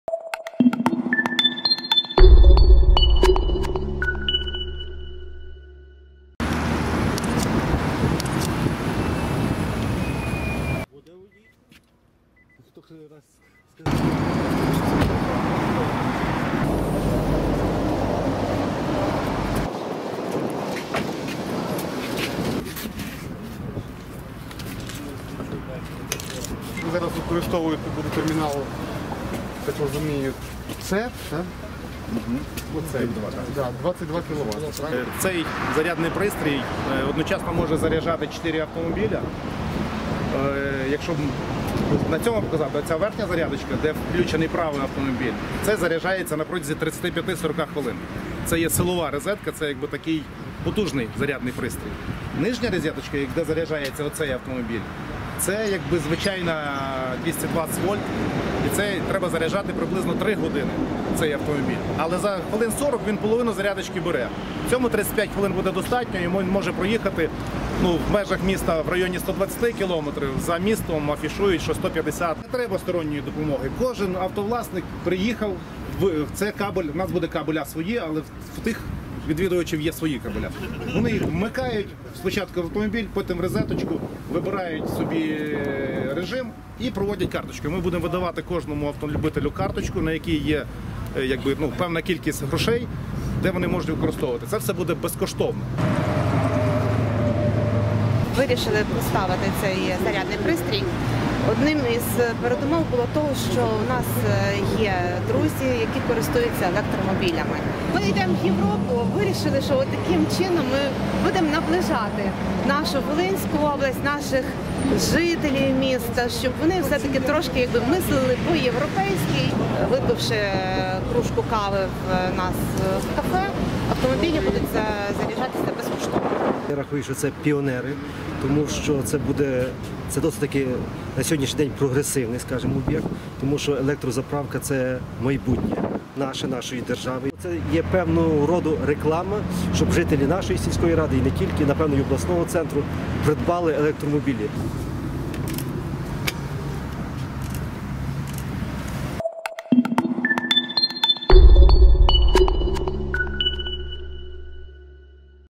Поблизу Луцька відкрили зарядну станцію для електрокарів
17 лютого у селі Рованці (Луцький район) на об'їзній дорозі відкрили зарядну станцію для електромобілів. Обладнання для станції виготовлене в Україні, має усі можливі режими зарядки: два порти на 3 кВт, один - 22 кВт та ще один - на 11 кВт. Тут водночас  можуть заряджатися чотири автомобілі, електроскутери чи електровелосипеди.